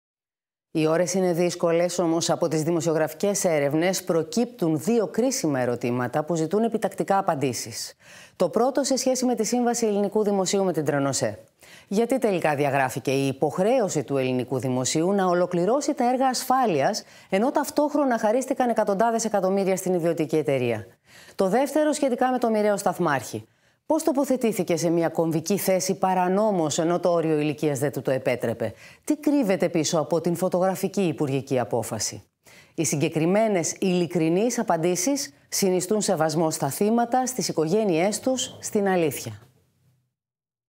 Π. Τσαπανίδου: Η κυβέρνηση να δώσει απαντήσεις για τα έργα ασφάλειας και την πρόσληψη του σταθμάρχη
On camera δήλωση της εκπροσώπου Τύπου του ΣΥΡΙΖΑ-Προοδευτική Συμμαχία